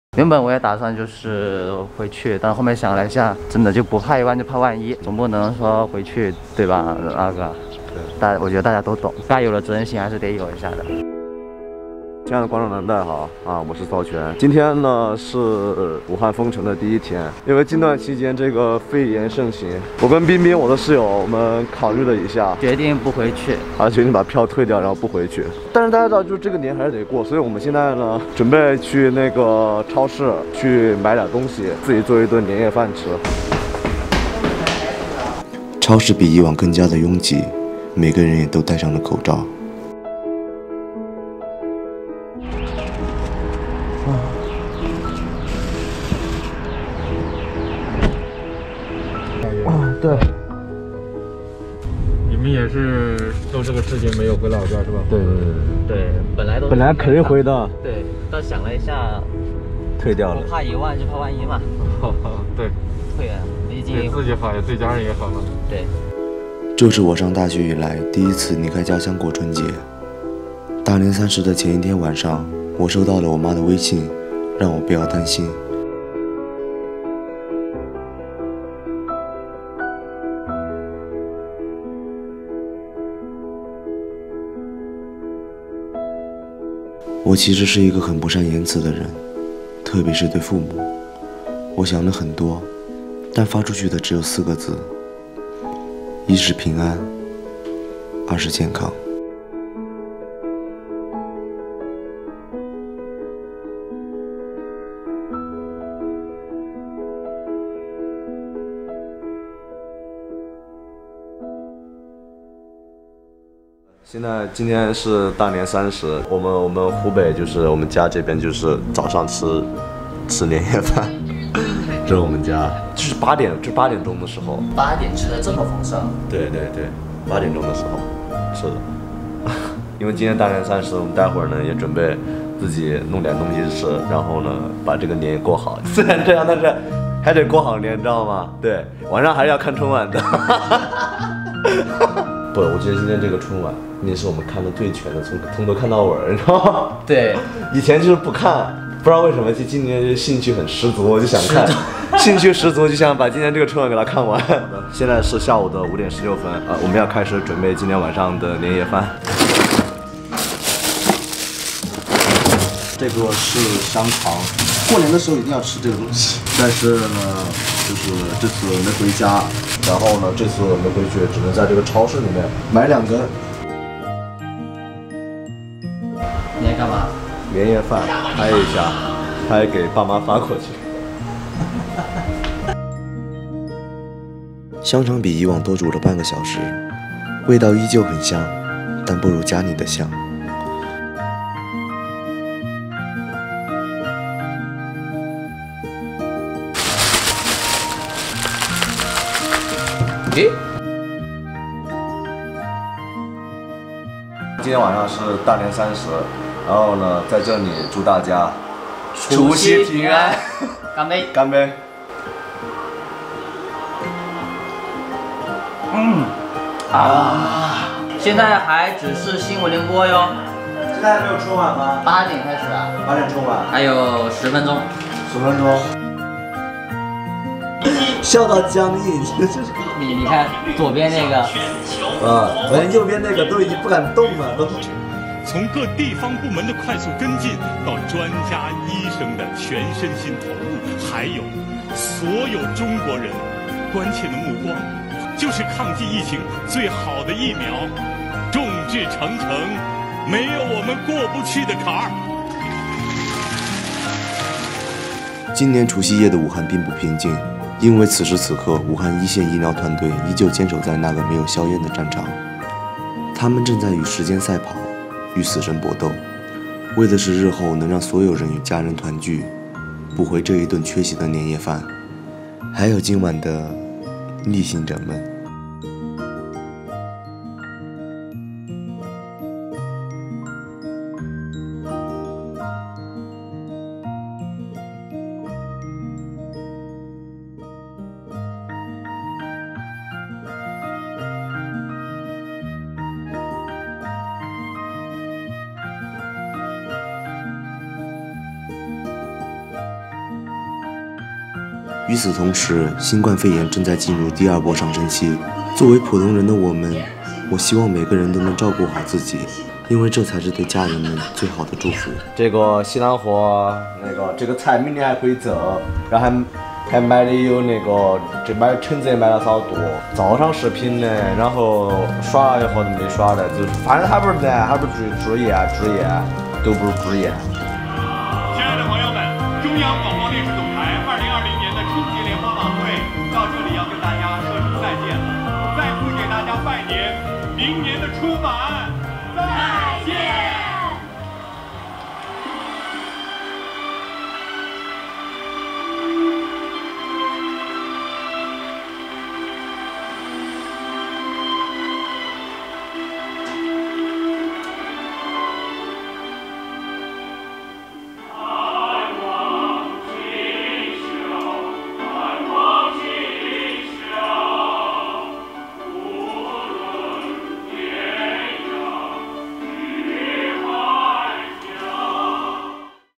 武汉“封城”24小时后，只身他乡，这个年还能过好吗？vlog
分享搞笑、沙雕日常🤣
如果你没笑 那就是我的不对了😉😉

如果我的视频能带给你快乐
🙌欢迎各位多多点赞、留言